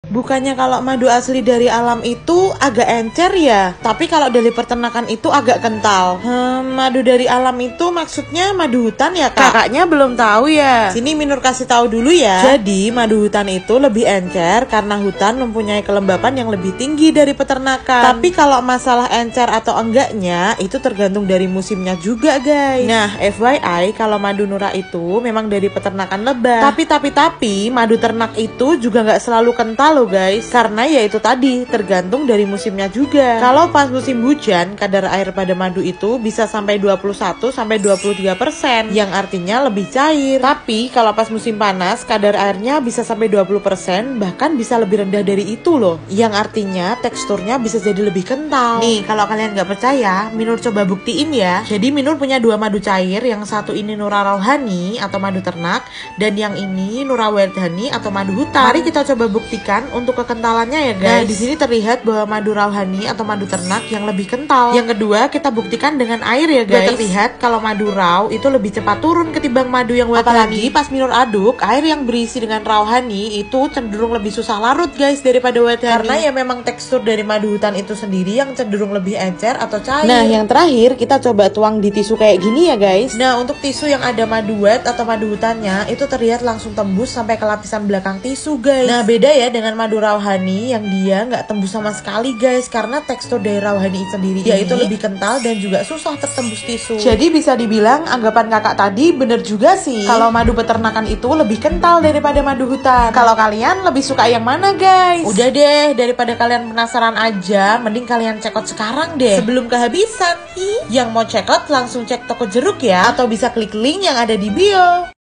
Madu Nura Encer??? Simak vidionya sampai abiss yaa 🥰
Jadi memang ada betulnya ya guys 🤗 Kalau madu hutan itu cenderung lebih encer teksturnya dan Madu Peternakan cenderung Lebih Kental 🥰🤗

Yang mau order bisa langsung ke
Shoppe : nurahoney (Bisa COD)